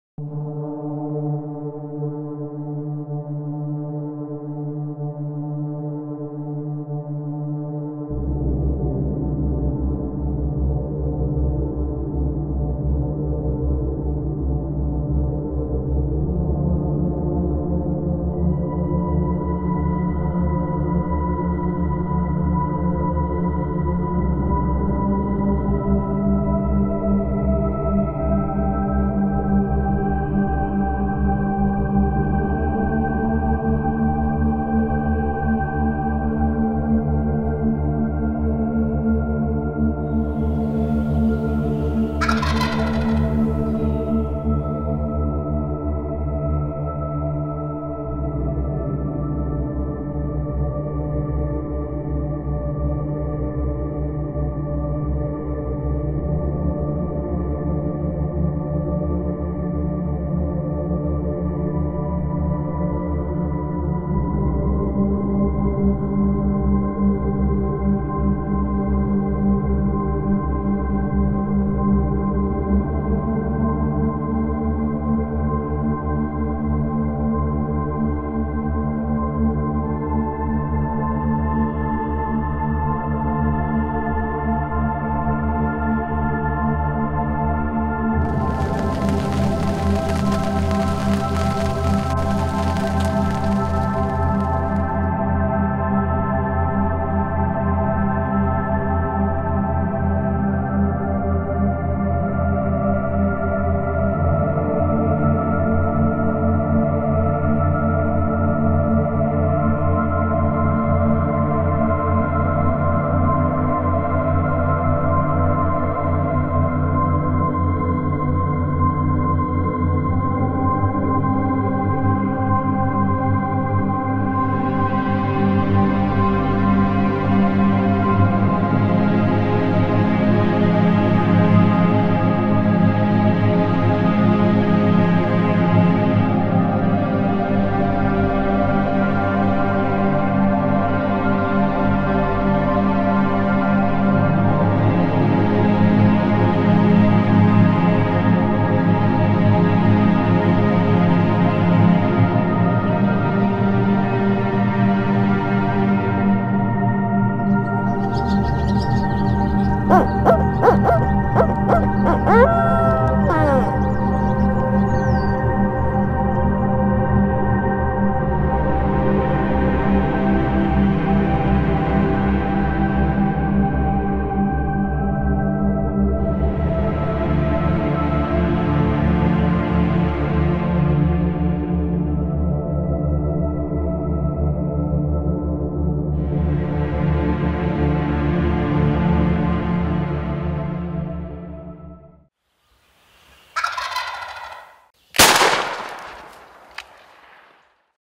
Spring Turkey Feature 2018 | Tainted Red Outdoors
The 2018 Wisconsin turkey season was a tough one! The hunt started out with a blizzard that dropped 30 inches of snow on the ground, and ended in 90 degree heat with swarming mosquitoes. Even with the tough hunting conditions, the team managed to get 3 turkeys on the ground this year!